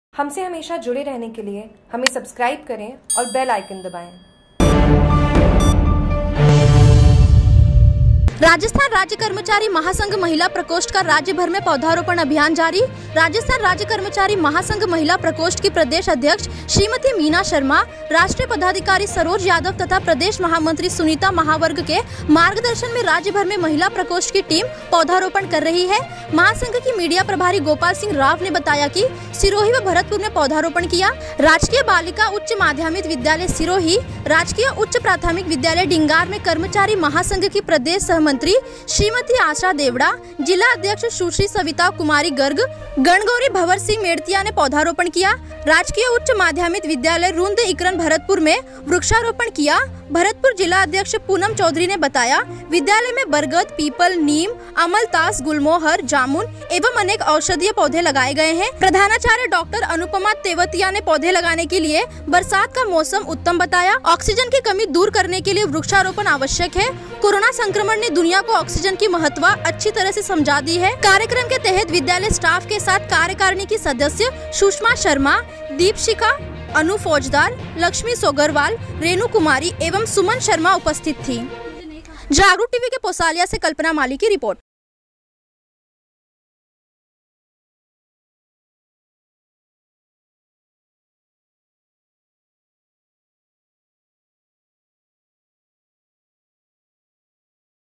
राजस्थान राज्य कर्मचारी महासंघ महिला प्रकोष्ठ का राज्य भर में पौधारोपण अभियान जारी
राजस्थान राज्य कर्मचारी महासंघ महिला प्रकोष्ठ का राज्य भर में पौधारोपण अभियान जारी ◼️सिरोही -  राजस्थान राज्य कर्मचारी महासंघ महिला प्रकोष्ठ की प्रदेश अध्यक्ष श्रीमती मीना शर्मा ,राष्ट्रीय पदाधिकारी सरोज यादव तथा प्रदेश महामंत्री सुनीता महावर के मार्गदर्शन में राज्य भर में महिला प्रकोष्ठ की टीम पौधारोपण कर रही है । महासंघ की मीडिया प्रभारी गोपाल सिंह राव ने बताया कि सिरोही व भरतपुर में पौधारोपण किया । राजकीय बालिका उच्च माध्यमिक विद्यालय सिरोही , राजकीय उच्च प्राथमिक विद्यालय डिंगार में कर्मचारी महासंघ की प्रदेश सहमंत्री श्रीमती आशा देवडा ,जिला अध्यक्ष सुश्री सविता कुमारी गर्ग , गणगौरी , भंवरसिंह मेडतिया ने पौधारोपण किया ।  राजकीय उच्च माध्यमिक विद्यालय रुंध इकरन भरतपुर में  वृक्षारोपण किया।भरतपुर जिला अध्यक्ष पूनम चौधरी ने बताया विद्यालय में बरगद, पीपल ,नीम, अमलतास ,गुलमोहर ,जामुन एवं अनेक औषधीय पौधे लगाए गए। प्रधानाचार्य डॉक्टर अनुपमा तेवतिया ने पौधे लगाने के लिए बरसात का मौसम उत्तम बताया। ऑक्सीजन की कमी को दूर करने के लिए वृक्षारोपण आवश्यक है।कोरोना संक्रमण ने दुनियां को आक्सीजन की महत्ता अच्छी तरह से समझा दी है ।कार्यक्रम के तहत विद्यालय स्टाफ के साथ कार्यकारिणी की सदस्य सुषमा शर्मा, दीपशिखा, अनु फौजदार ,लक्ष्मी सोगरवाल, रेनू कुमारी एवं सुमन शर्मा उपस्थित थी । पोसालिया से कल्पना माली रिपोर्टर